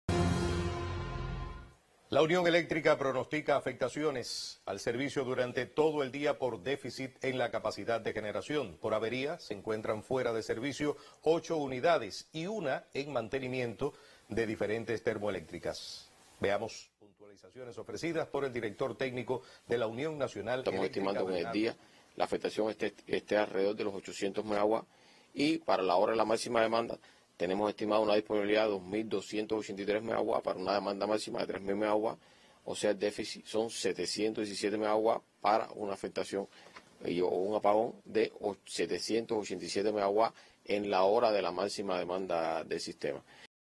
La Unión Eléctrica de Cuba informa hoy nuevas afectaciones.
La UniónEléctrica de Cuba pronostica afectaciones al servicio durante todo el día por déficit en la capacidad de generación. Por avería se encuentran fuera de servicio 8 unidades y una en mantenimiento, de diferentes Termoeléctricas.